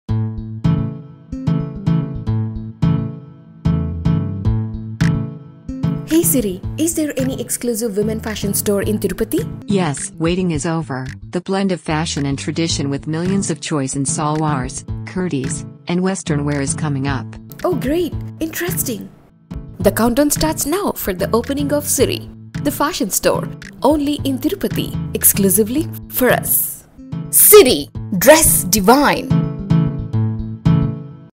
Siri Dress Divine Exclusive Women's Fashion Store in Tirupati | Actress Lavanya Tripathi
Siri Dress Divine Exclusively For Ladies with a multitude of ridiculously Cool Fast Fashion Apparel & Accessories. Shop From Our Store launching soon in Tirupati. Andhra Pradesh.

Follow us @siridressdivine